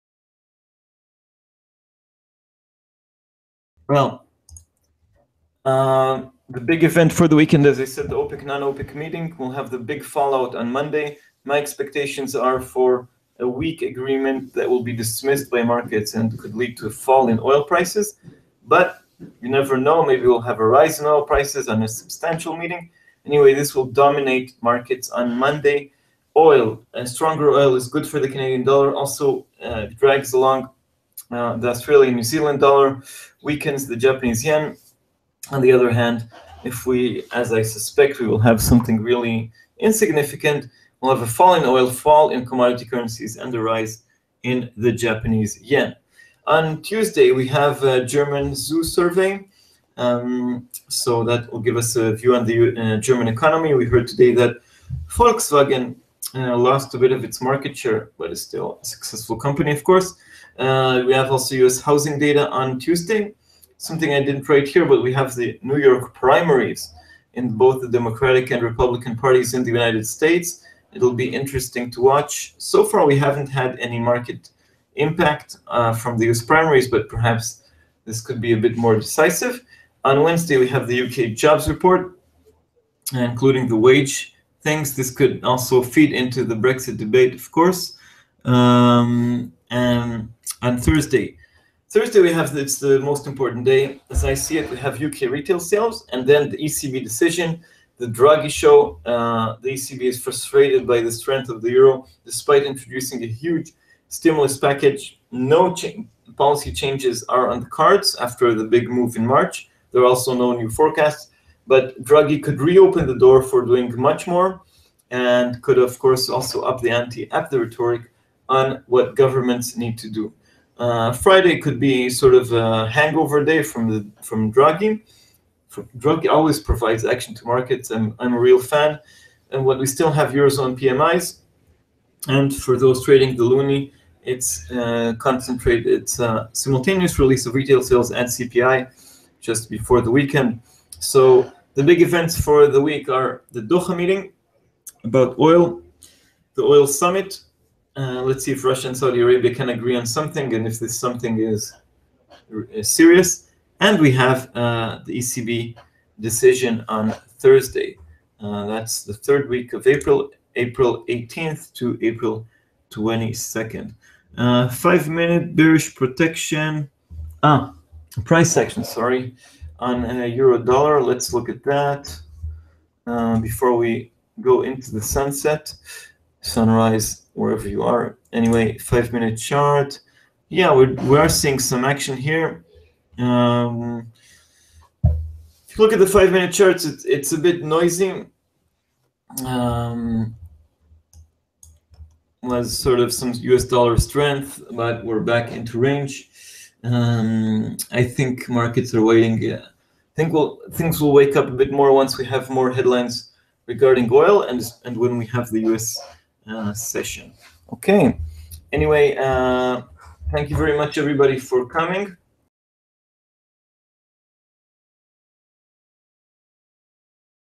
Yohay's Preview: The Draghi show, potential oil sell-off and more
The new week commences with a reaction to the OPEC-Non-OPEC meeting in Doha. Will the leaders agree on a substantial agreement to freeze production? A disappointment could result in a sell off. Another big event of the week is the ECB meeting: while no change is expected, Draghi could cast a dark cloud and try to weigh on the euro. Also watch out for UK employment and US housing starts, but oil and the euro could certainly lead all the rest.